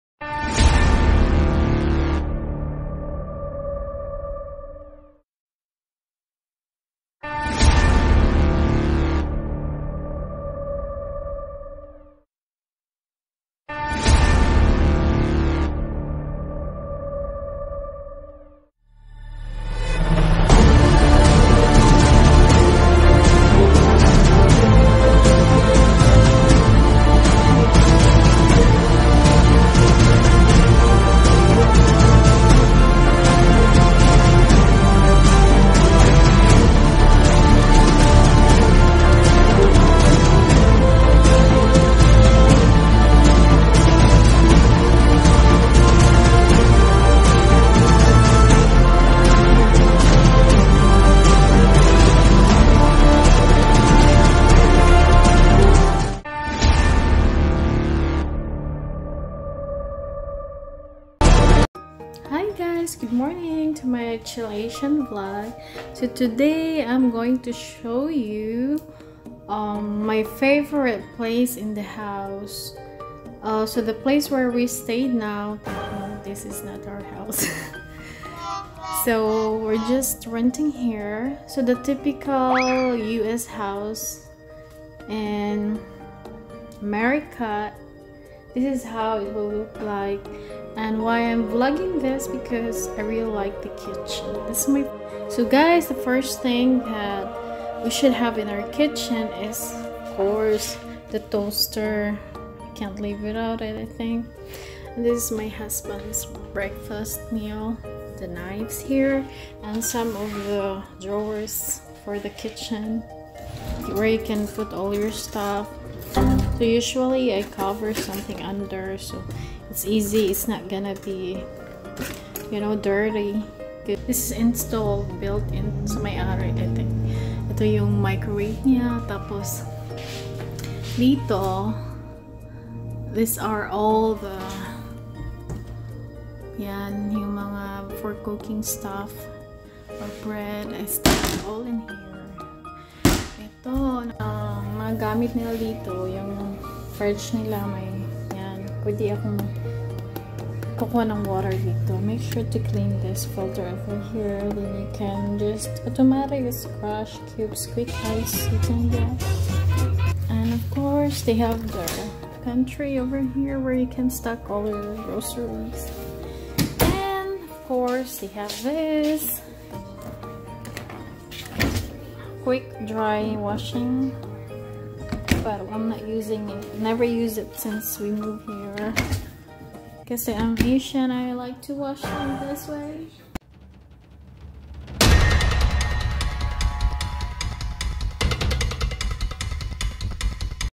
The "Kitchen" is my favorite place in the house!🤪 I mean in every house!Haha
I'm showing you what's my favorite place in the house. I spend most of my time in here these days! whenever I'm hungry,thirsty and need some snacks! I go to the kitchen as often as you can imagine!!!🤣